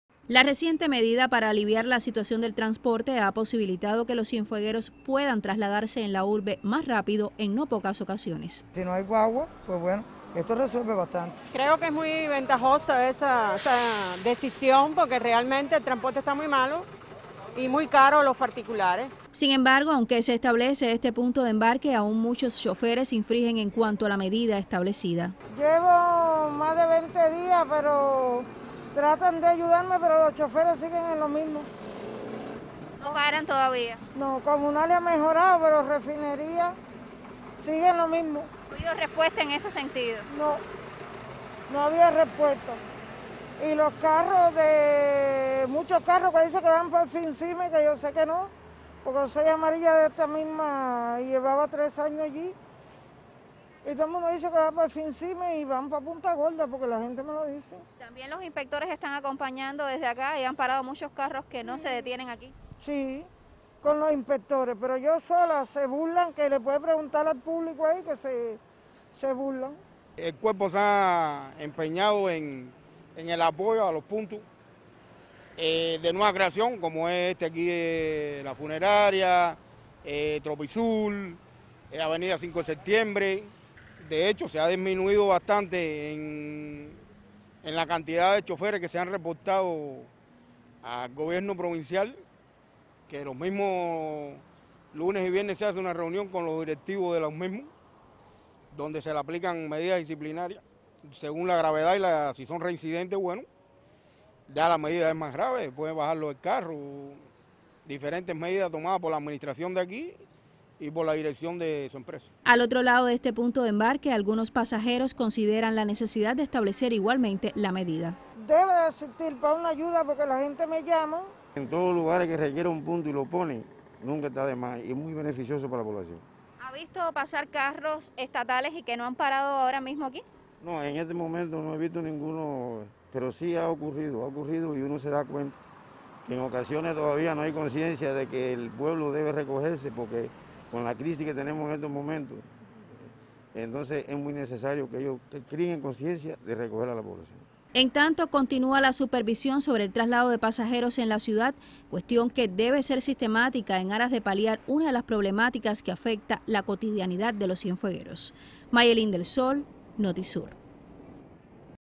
Nuevos puntos de embarque ¿beneficio total?
Con la activación de nuevos puntos de embarque en la ciudad de Cienfuegos se aprecian beneficios en el traslado de pasajeros, aunque todavía es insuficiente para la población.